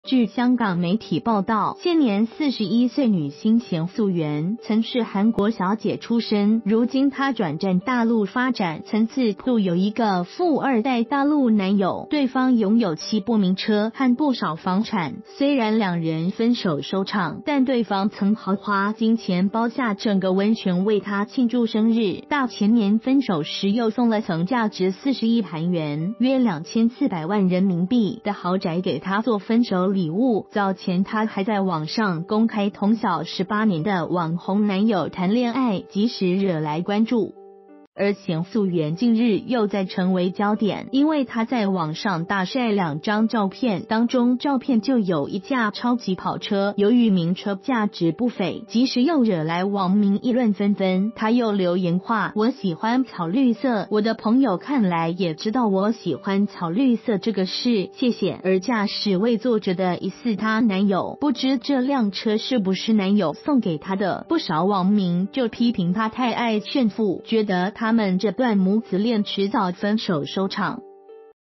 收两千万豪宅做分手礼物 41岁女星晒车被骂炫富
發表評論評論如下。